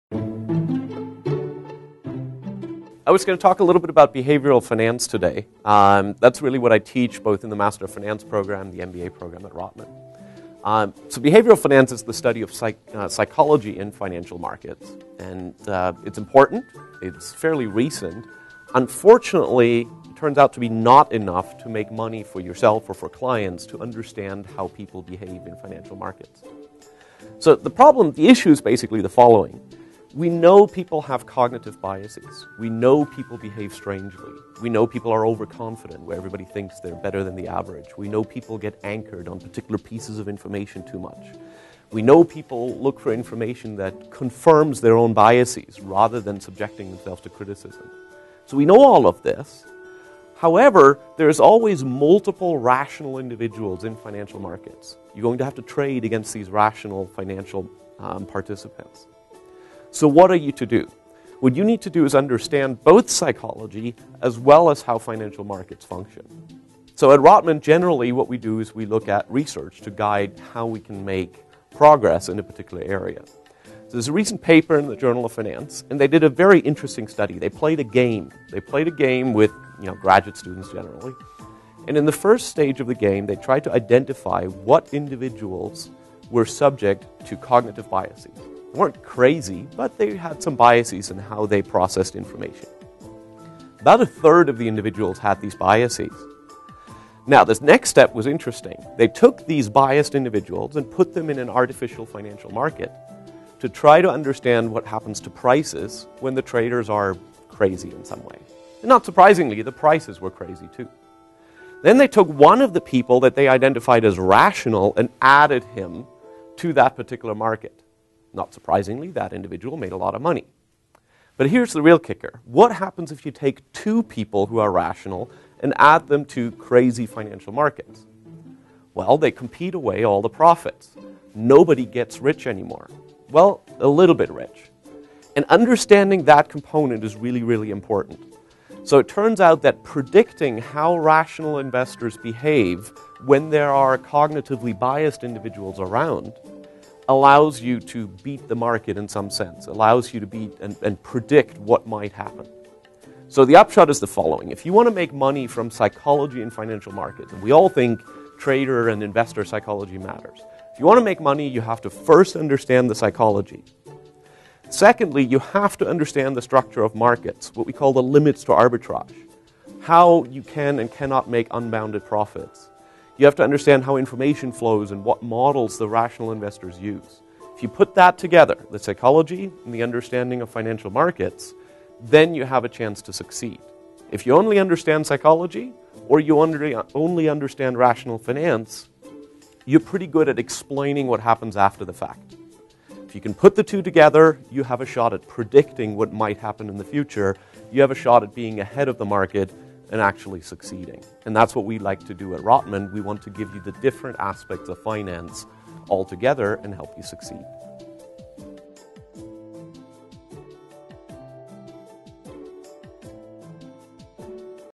Rotman Professor Jan Mahrt-Smith on Behavioural Finance
Professor Jan Mahrt-Smith of the Master of Finance and MBA programs at the Rotman School examines the role that psychology plays in financial markets. The Rotman School of Management is the business school at the University of Toronto in Toronto, Canada.